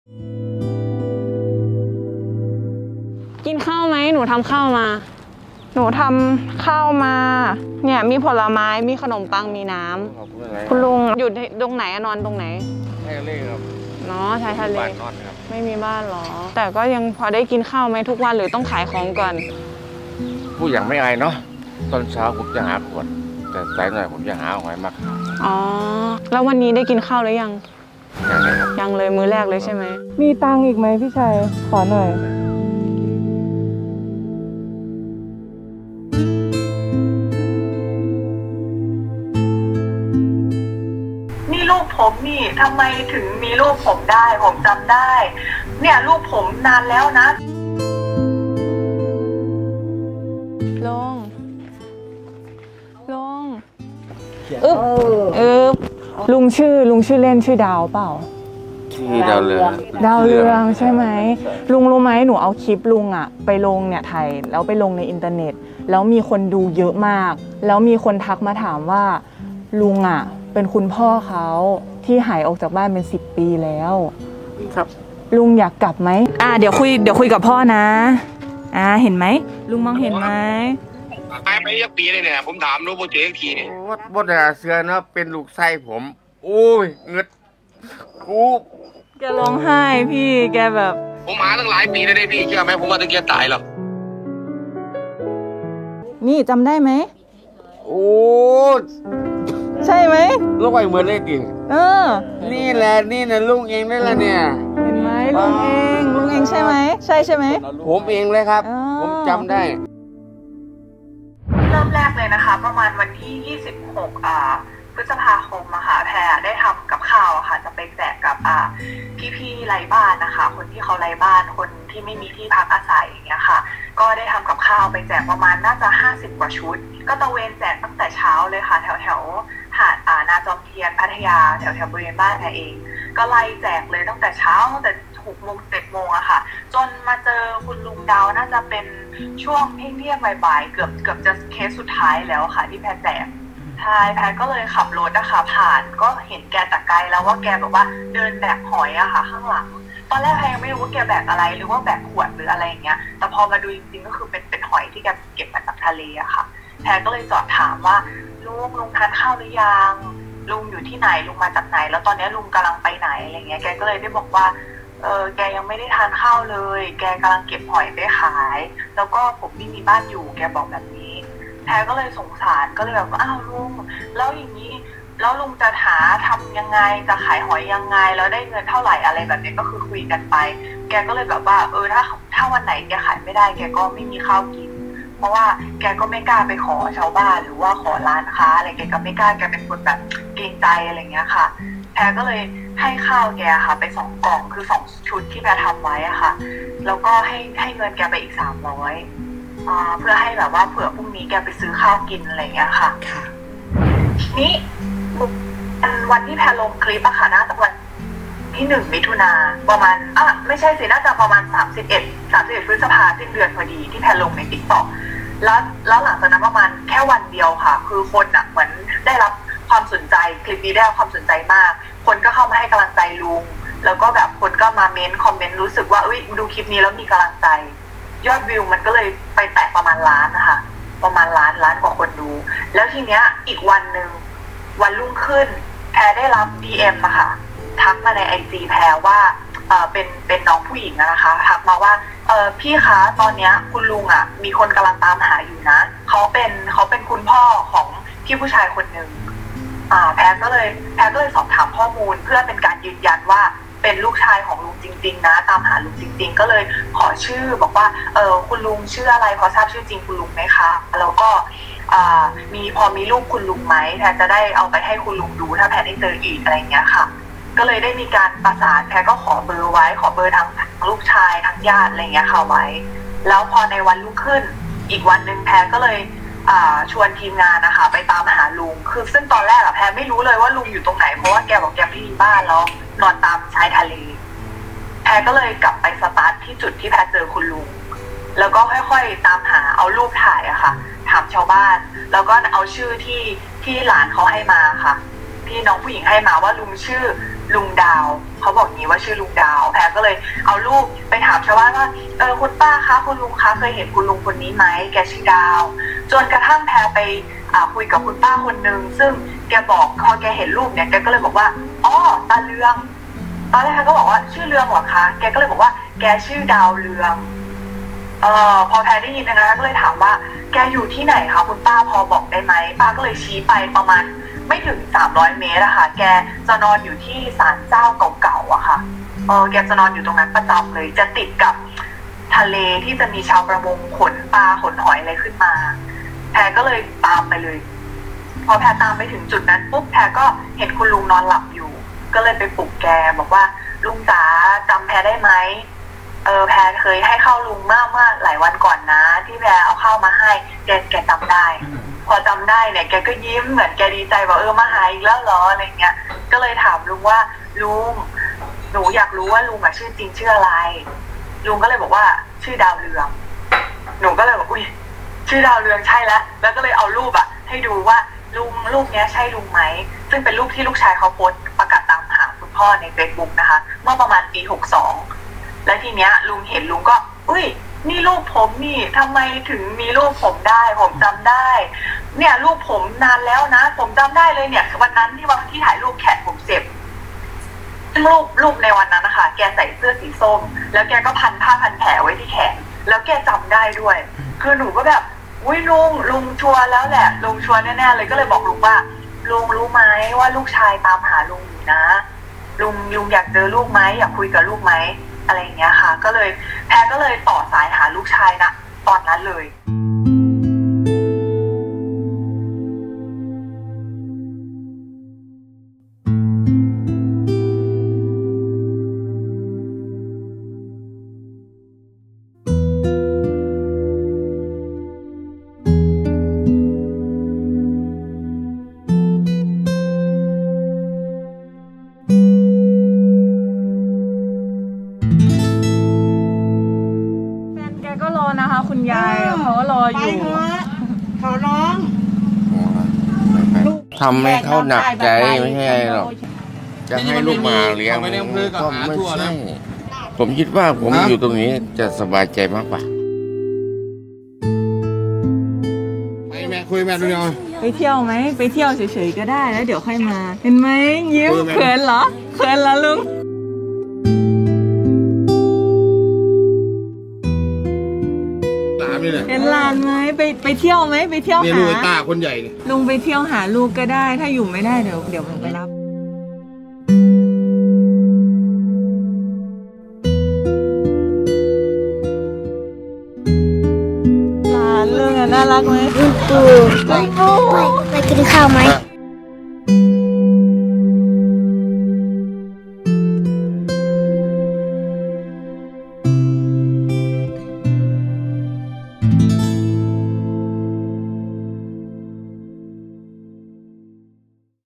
แพร R1 ลงคลิปช่วยลุงไร้บ้าน พ่อลูกพลัดพราก 15 ปีพบหน้ากัน : Khaosod TV
แพร R1 เผยนาทีบีบหัวใจ ลงคลิปช่วยลุงไร้บ้าน ทำพ่อลูกพลัดพราก 15 ปี ได้พบหน้ากัน